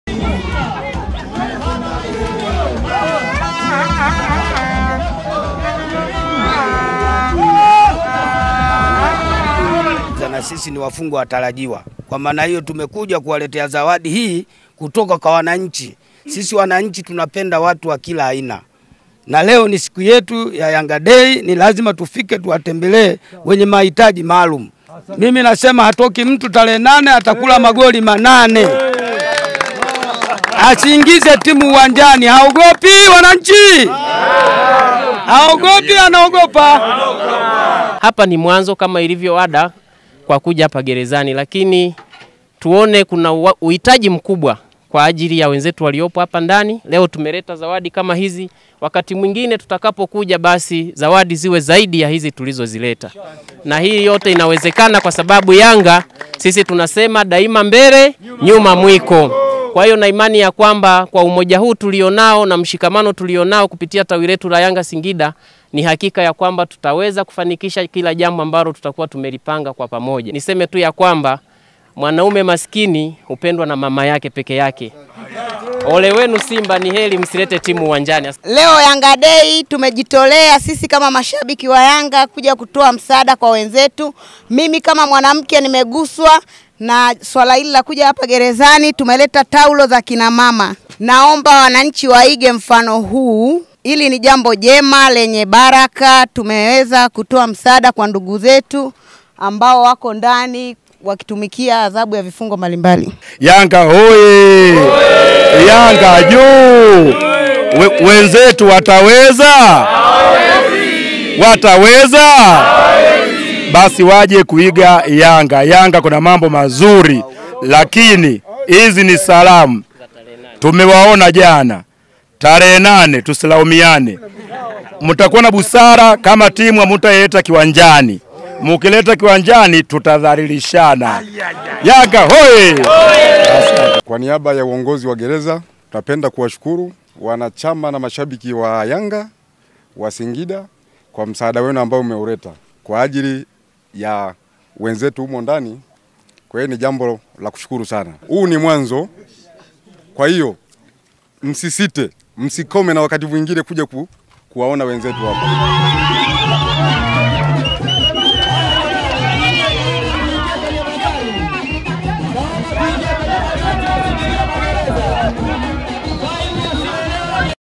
Mashabiki Wa Yanga Singida Hawanadogo😁, Watoa Onyo Kwa Simba Kuelekea Derby August 8
Mashabiki wa Yanga mkoani Singida katika kuadhimisha kilele Cha siku ya mwananchi hii leo wametembelea gereza la wilaya la Singida na kutoka misaada ya vitu mbalimbali kwa Lengo la kuwafariji na kudumisha umoja na mshikamano